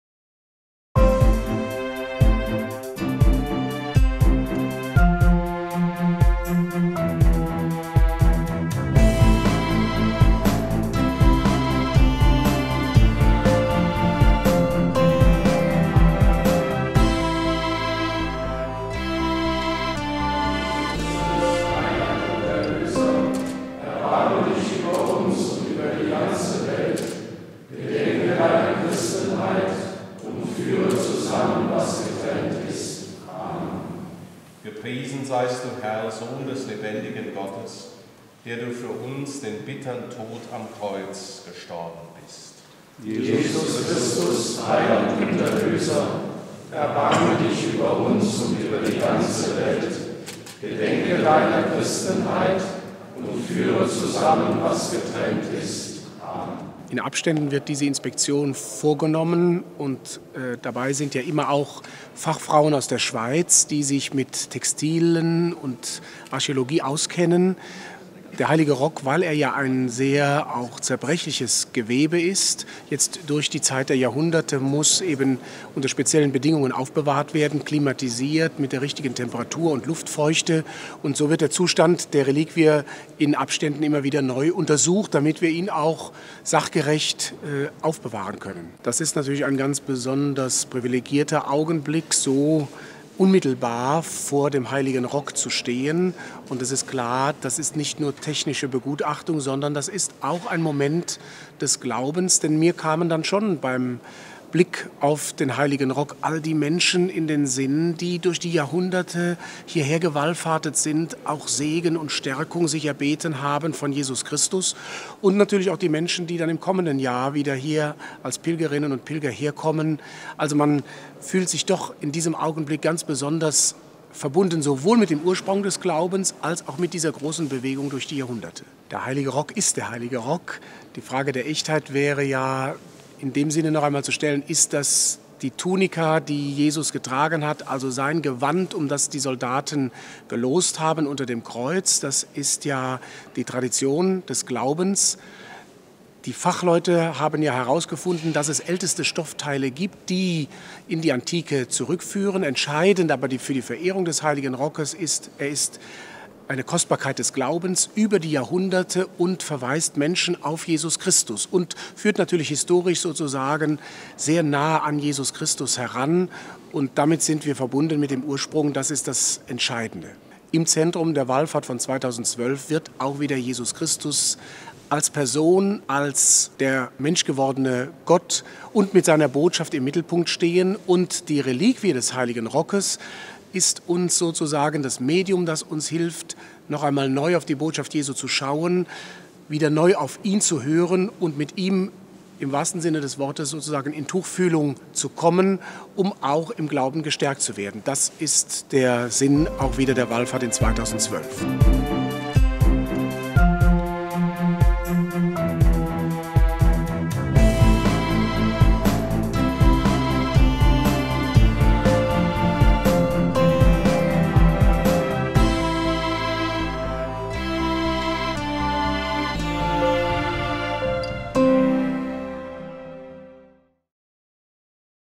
Mit Ihm auf Tuchfühlung
"... und führe zusammen, was getrennt ist." Vom 13. April bis zum 13. Mai 2012 findet zum ersten Mal in diesem Jahrtausend in Trier die Wallfahrt zum Heiligen Rock statt. Um den Zustand der Tunika zu begutachten, wird sie alle paar Jahre inspiziert von der Domgeistlicjeit und Wissenschaftlern. Das ist mehr als ein technischer Akt, sagt der Trierer Bischof Ackermann, der diese Inspektion Ende Februar 2011 durchführte.